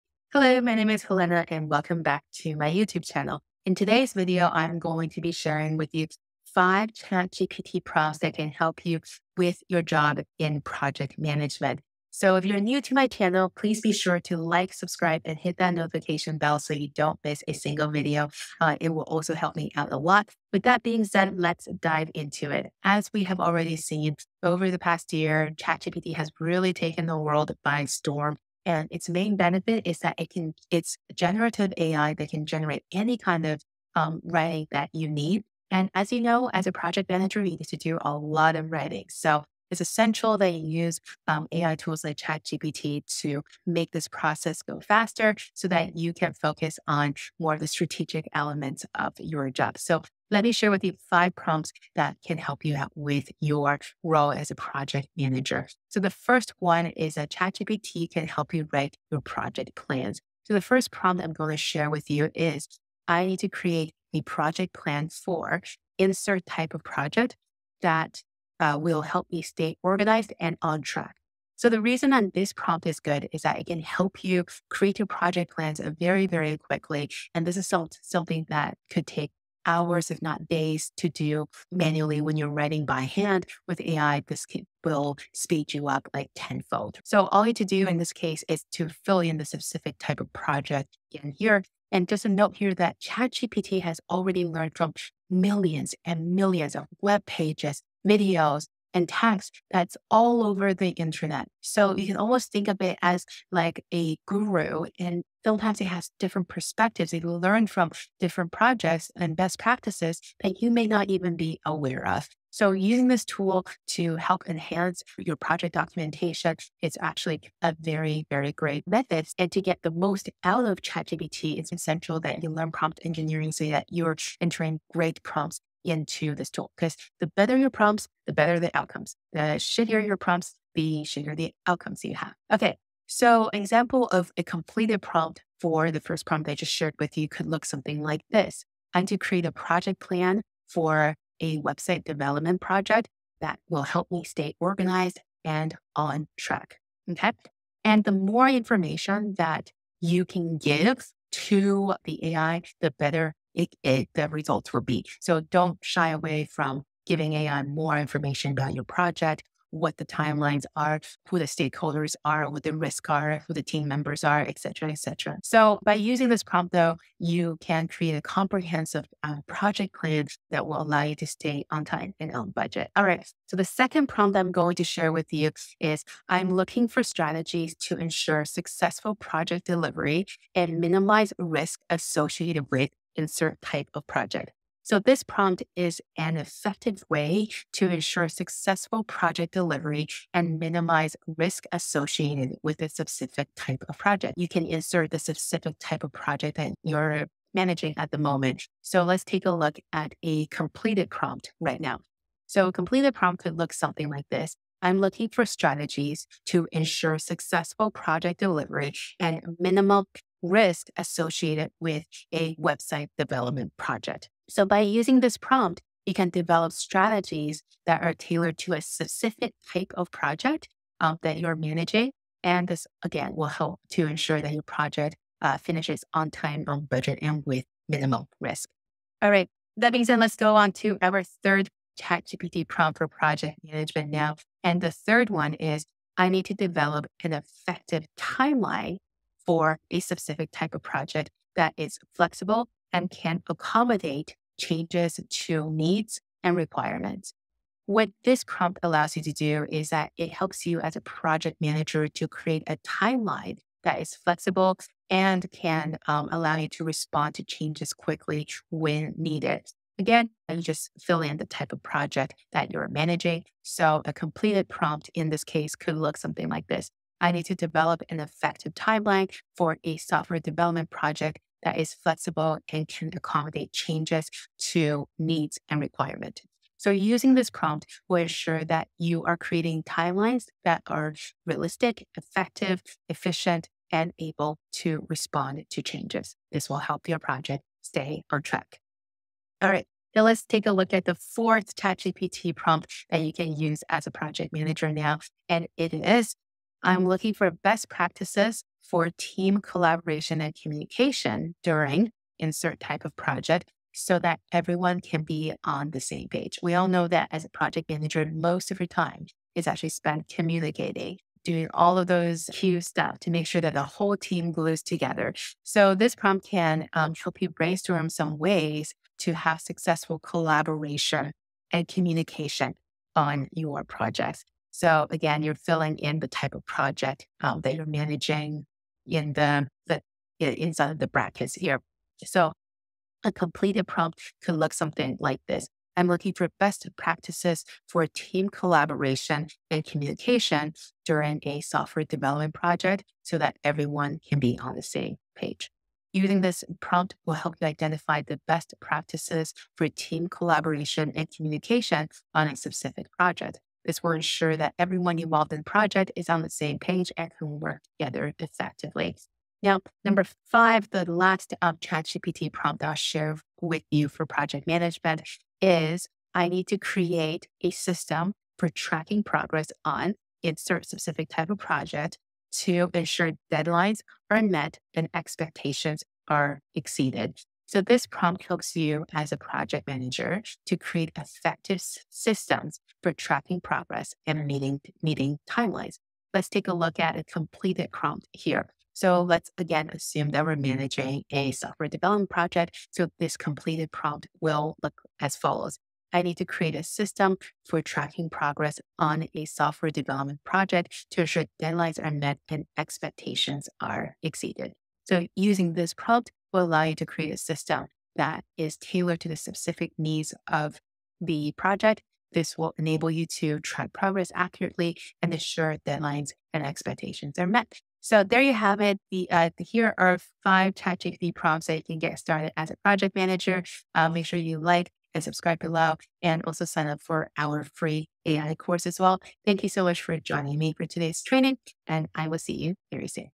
ChatGPT Prompts for Project Management
In this video, we cover 5 ChatGPT Prompts for Project Management. As a project manager, you probably need to do a lot of writing, and thankfully, now you can use AI to help you out. Here are 5 ways that AI can help you cut down on your project planning and documentation work.

📈 Learn AI in 5 mins a week w/my free AI newsletter